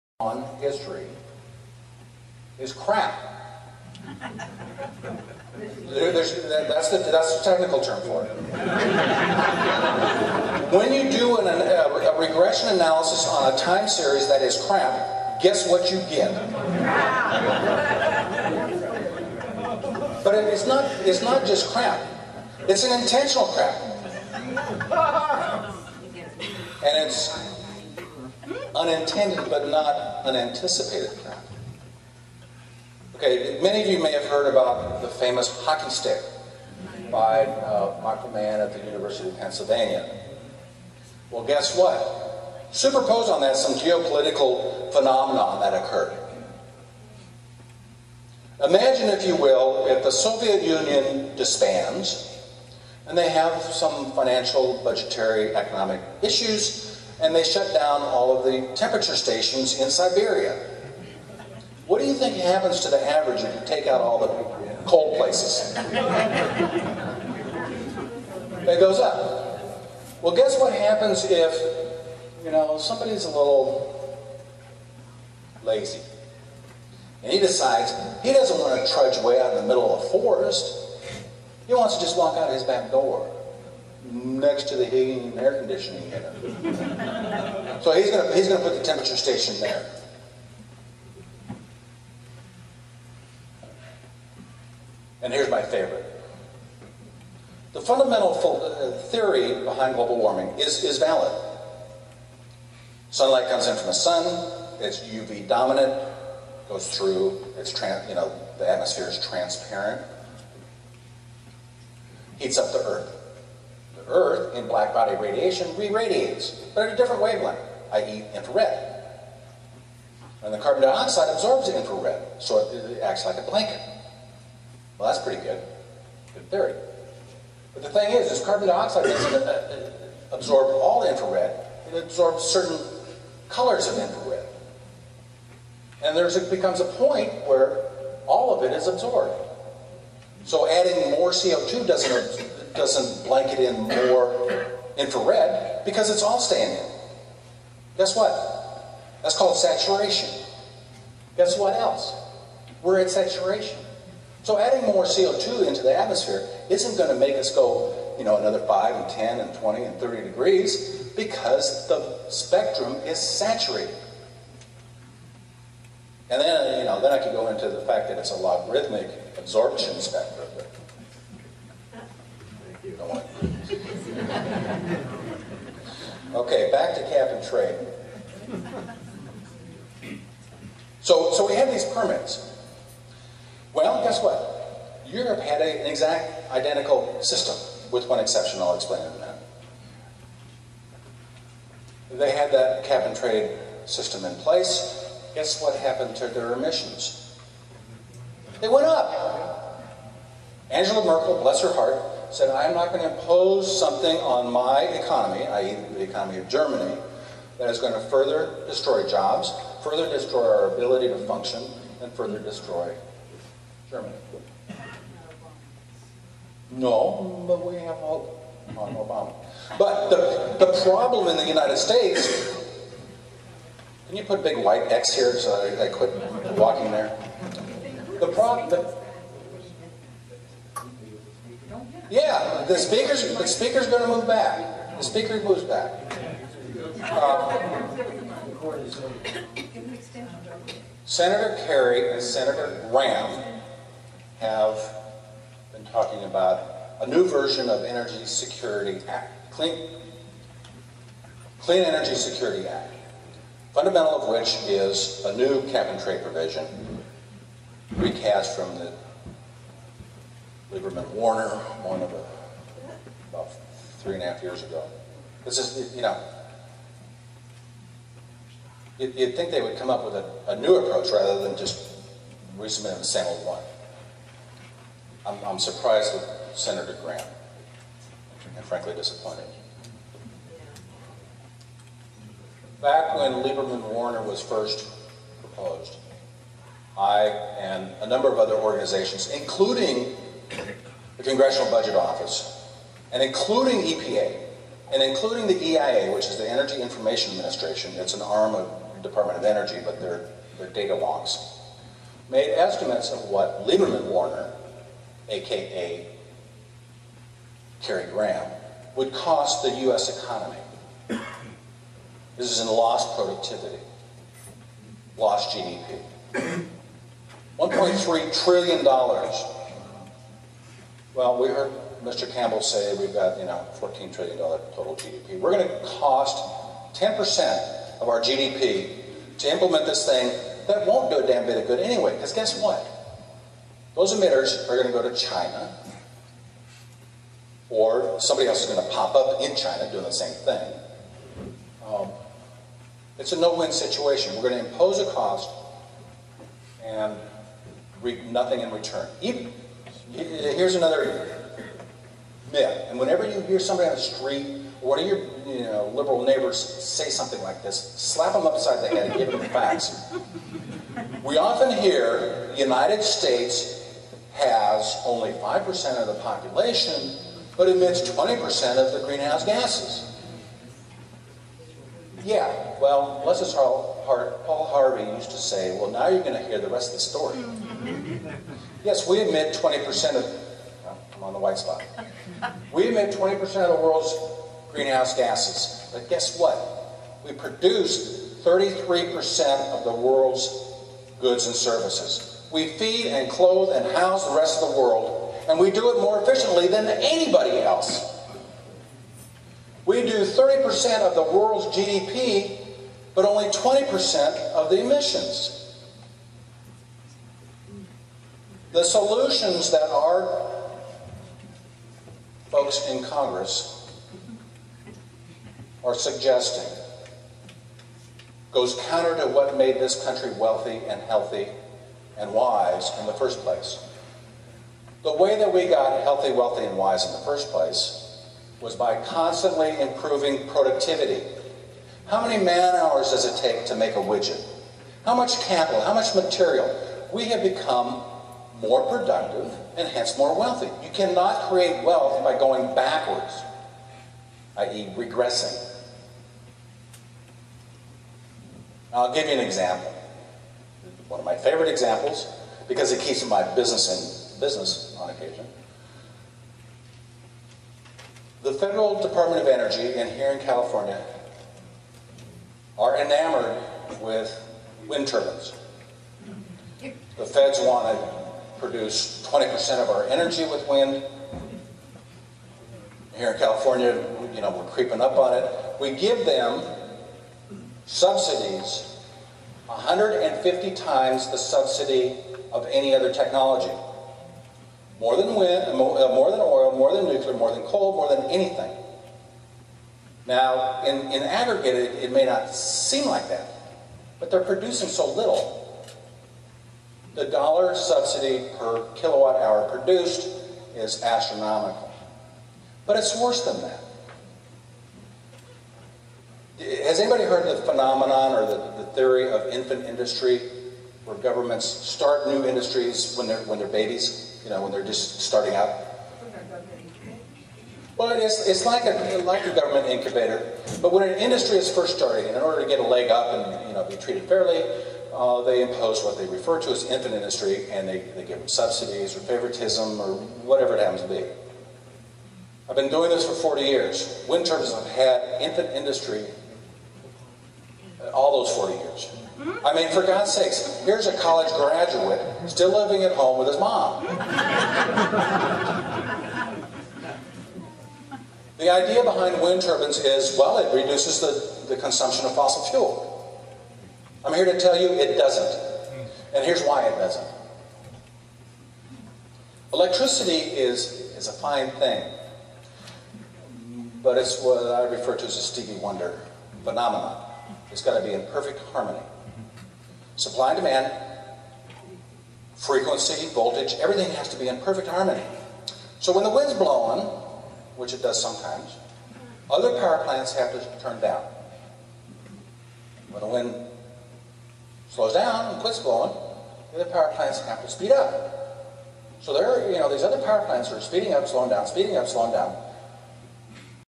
Thomas Tanton ~ 3 of 7
The Conservative Forum.  Thomas Tanton, speaking on Cap & Trade bill proposed for our nation and economy, and what it will do to businesses.